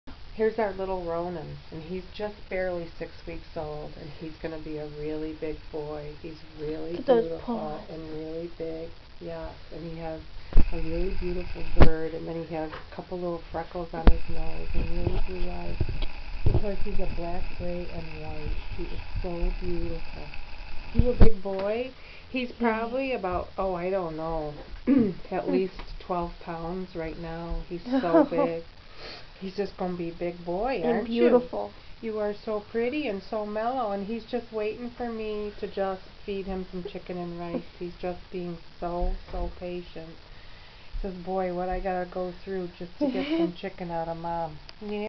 Ronan...at six weeks!!!
Ronan is a classic black and white with very blue eyes. He has a touch of grey ticking...that is very lovely. He is shy and going to be a big boy. he has a few freckles on his nose that are adorable.